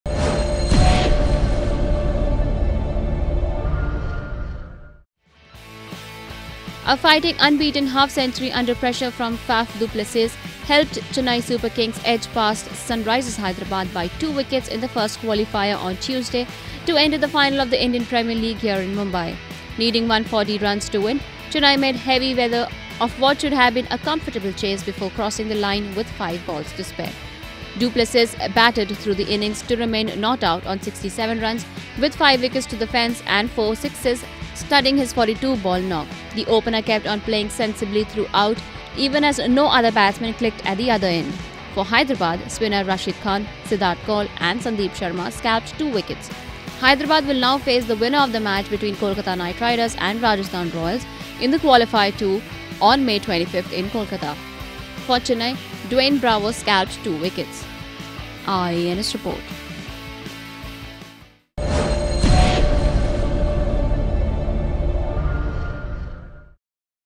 IPL 2018 | Chennai edge out Hyderabad in thriller to enter final
A fighting unbeaten half-century under pressure from Faf du Plessis helped Chennai Super Kings edge past Sunrisers Hyderabad by two wickets in the first qualifier on Tuesday to enter the final of the Indian Premier League here in Mumbai.

Brief scores: Sunrisers Hyderabad: 139/7 in 20 overs (Carlos Brathwaite 43 not out; Dwayne Bravo 2/25) against Chennai Super Kings: 140/8 in 19.1 overs (Du Plesis 67; Rashid Khan 2/12)